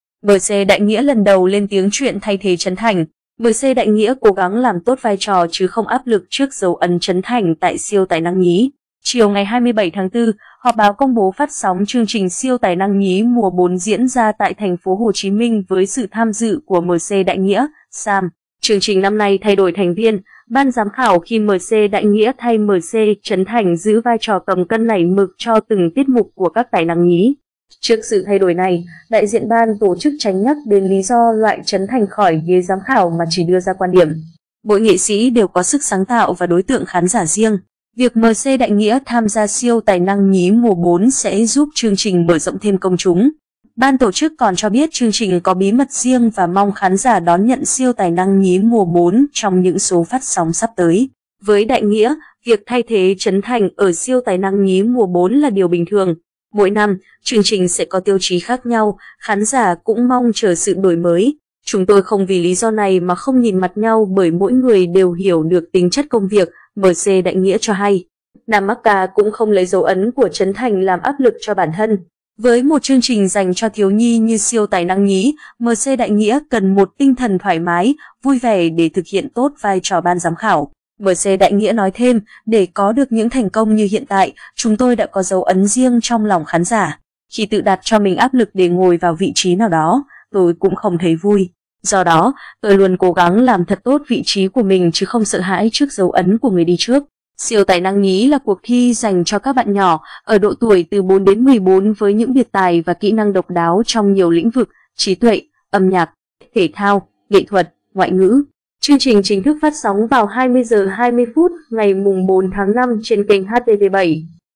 MC Đại Nghĩa lần đầu lên tiếng chuyện thay thế Trấn Thành
MC Đại Nghĩa cố gắng làm tốt vai trò chứ không áp lực trước dấu ấn Trấn Thành tại Siêu tài năng nhí.BTC còn cho biết chương trình có bí mật riêng và mong khán giả đón nhận Siêu tài năng nhí mùa 4 trong những số phát sóng sắp tới.
Nam MC cũng không lấy dấu ấn của Trấn Thành làm áp lực cho bản thân. Với một chương trình dành cho thiếu nhi như Siêu tài năng nhí, MC Đại Nghĩa cần một tinh thần thoải mái, vui vẻ để thực hiện tốt vai trò BGK.
Chiều 27/4, họp báo công bố phát sóng chương trình Siêu tài năng nhí mùa 4 diễn ra tại TP.HCM với sự tham dự của MC Đại Nghĩa, Sam,… Chương trình năm nay thay đổi thành viên ban giám khảo khi MC Đại Nghĩa thay MC Trấn Thành giữ vai trò ''cầm cân nảy mực” cho từng tiết mục của các tài năng nhí.
MC Đại Nghĩa tại họp báo. Ảnh: Siêu tài năng nhí
Với Đại Nghĩa, việc thay thế Trấn Thành ở Siêu tài năng nhí mùa 4 là điều bình thường. “Mỗi năm, chương trình sẽ có tiêu chí khác nhau, khán giả cũng mong chờ sự đổi mới. Chúng tôi không vì lý do này mà không nhìn mặt nhau bởi mỗi người đều hiểu được tính chất công việc”, MC Đại Nghĩa cho hay.
MC Đại Nghĩa nói thêm: “Để có được những thành công như hiện tại, chúng tôi đã có dấu ấn riêng trong lòng khán giả. Khi tự đặt cho mình áp lực để ngồi vào vị trí nào đó, tôi cũng không thấy vui. Do đó, tôi luôn cố gắng làm thật tốt vị trí của mình chứ không sợ hãi trước dấu ấn của người đi trước”.
Siêu tài năng nhí là cuộc thi dành cho các bạn nhỏ ở độ tuổi từ 4 - 14 với những biệt tài và kỹ năng độc đáo trong nhiều lĩnh vực: trí tuệ, âm nhạc, thể thao, nghệ thuật, ngoại ngữ... Chương trình chính thức phát sóng vào 20h20 ngày 4/5 trên kênh HTV7.
Trước sự thay đổi này, đại diện BTC tránh nhắc đến lý do “loại” Trấn Thành khỏi ghế giám khảo mà chỉ đưa ra quan điểm: “Mỗi nghệ sĩ đều có sức sáng tạo và đối tượng khán giả riêng. Việc MC Đại Nghĩa tham gia Siêu tài năng nhí mùa 4 sẽ giúp chương trình mở rộng thêm công chúng”.